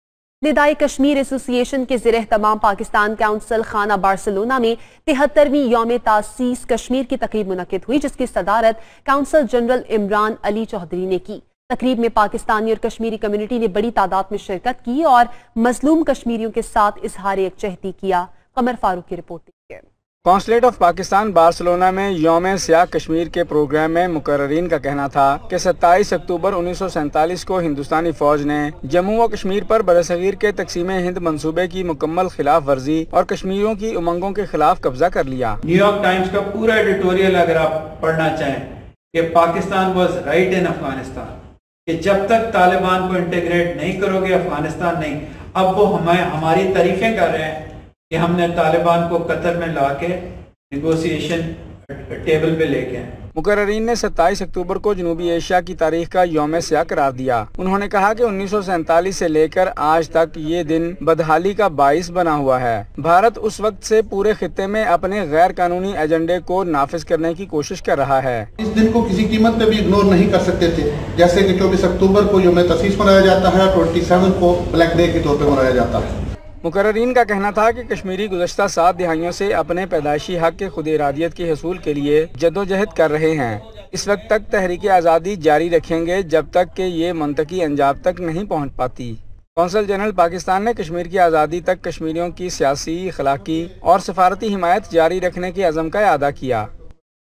73rd Kashmir Black Day Program organized by ندائے کشمیر Association at Pakistan Consulate Barcelona
73rd  Kashmir Black Day Program organized by ندائے کشمیر Association at Pakistan Consulate Barcelona
801۔۔26 اکتوبر 2020۔۔(120/07)ندائے کشمیر ایسوسی ایشن کے زیر اہتمام پاکستان قونصل خانہ بارسلونا میں 73ویں  یوم سیاہ کشمیر  کا پروگرام
رپورٹ قمرفاروق نمائندہ دنیا نیوزبارسلونا اسپین